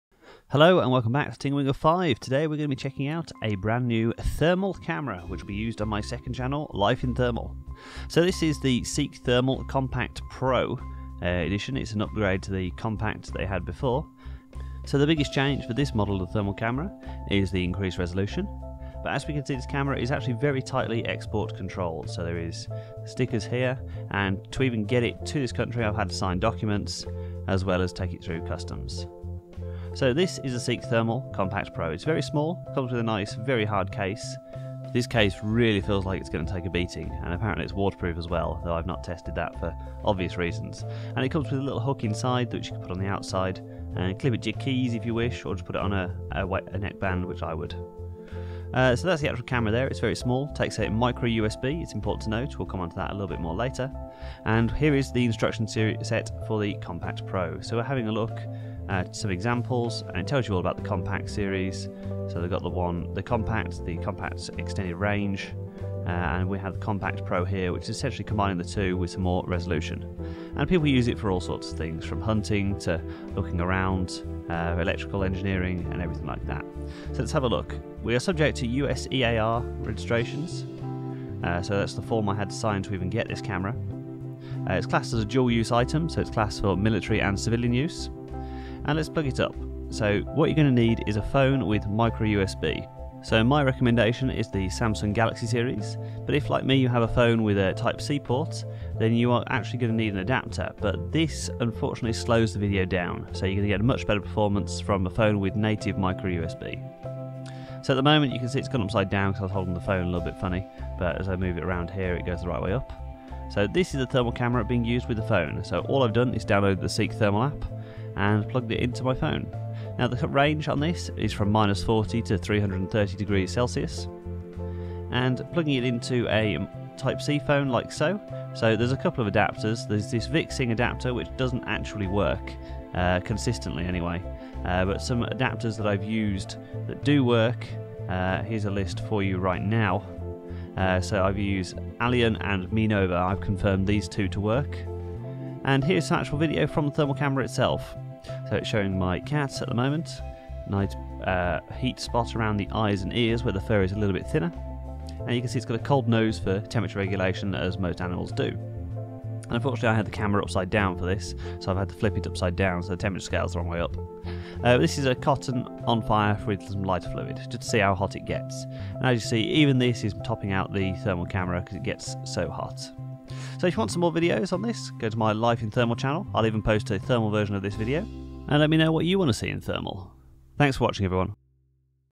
Seek Thermal Compact Pro Unboxing in Thermal
Seek Thermal Compact pro unboxing and setup shown in thermal.

Expect videos from this camera soon!

This is a thermography or a thermal video.

*Temperatures shown on videos may not be accurate and do not try this at home!